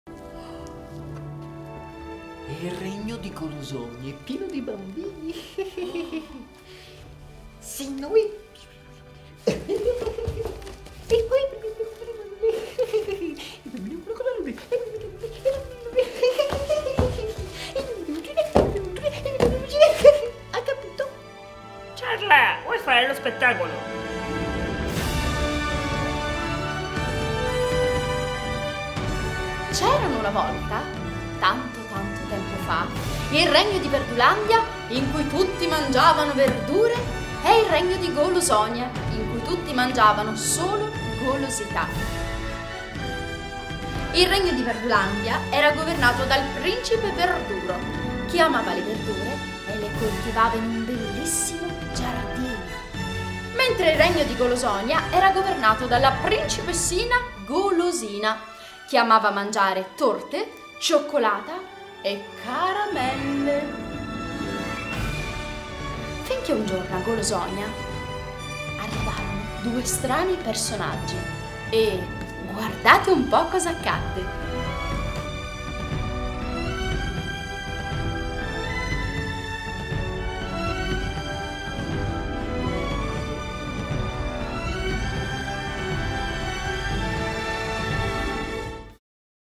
Il Regno di Golosonia - Trailer
Spettacolo sull'alimentazione
e con il sostegno di Telefono RosaAzzurro
Di e con: Chiara Anicito, Felice Ferrara ed Helga Micari